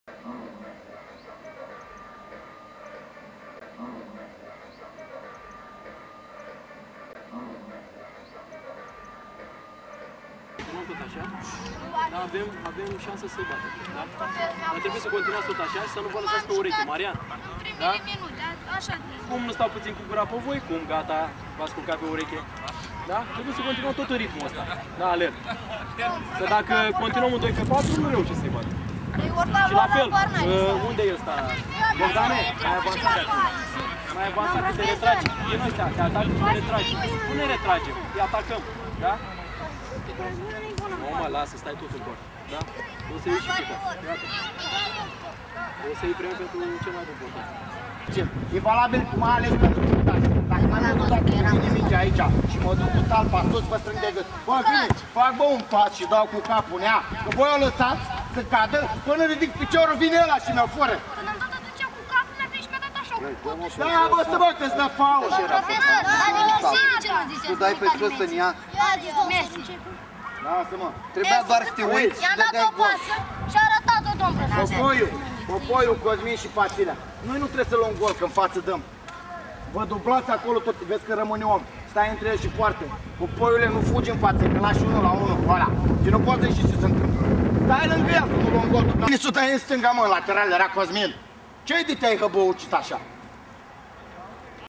DUNAREA GL-LPS GALATI 4-3 (2-2) PAUZA.mpg
MEMORIALUL NICOLAE BURCEA GRUPA 1998 EDITIA I GALATI 25-27 SEPT.2009 SFERTURI